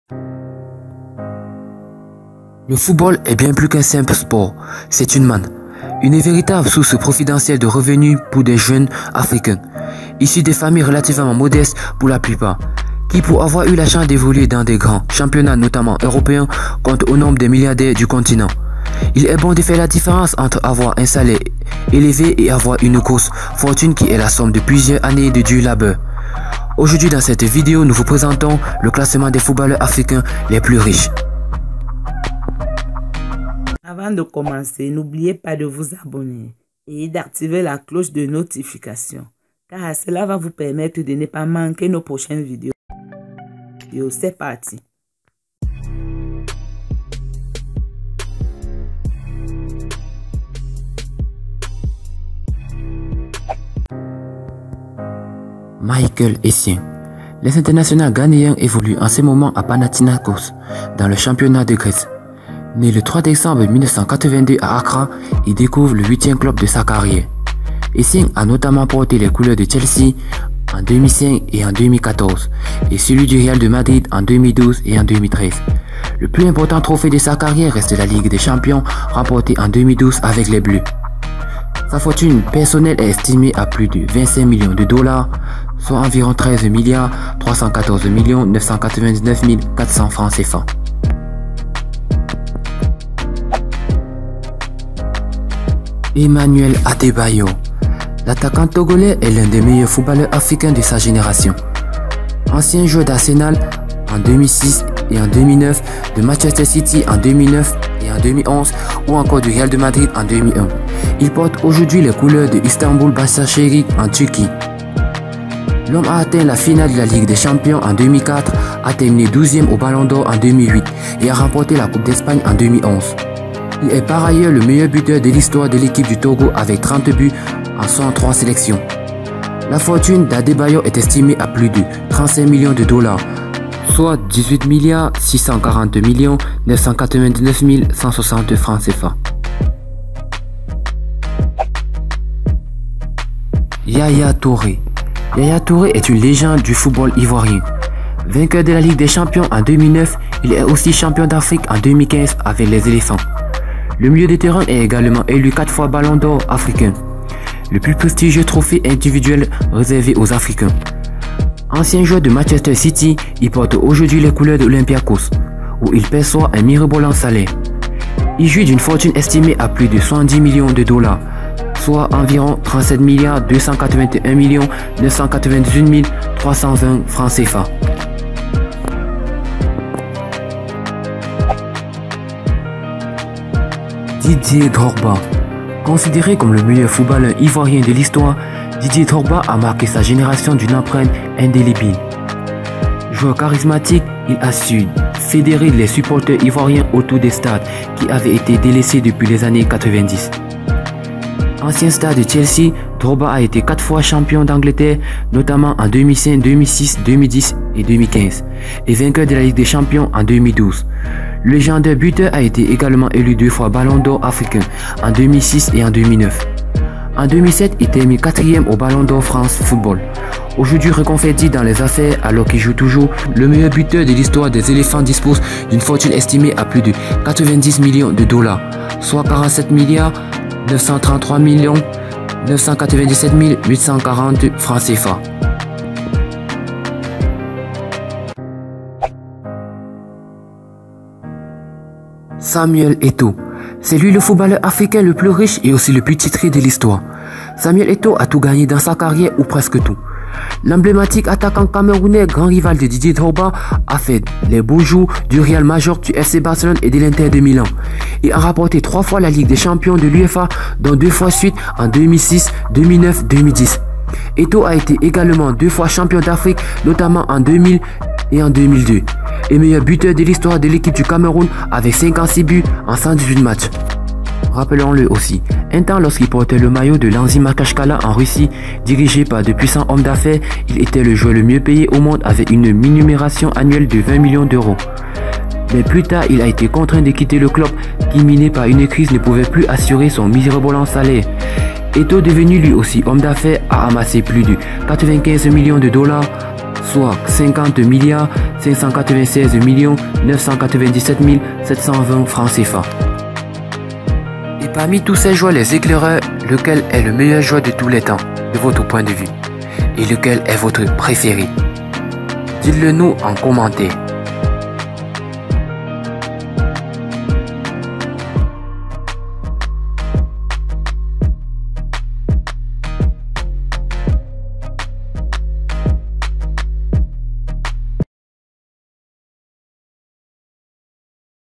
Les 5 footballeurs Africains les plus riches d'Afrique (2025) I La Torche du Monde
Le football est bien plus qu’un simple sport. C’est une manne. Une véritable source providentielle de revenue pour des jeunes africains, issus de familles relativement modestes pour la plupart qui, pour avoir eu la chance d’évoluer dans les grands championnats notamment européens, comptent au nombre des milliardaires du continent.Pour vous donner un aperçu de leur fortune. Voici les 5 footballeurs Africains les plus riches d'Afrique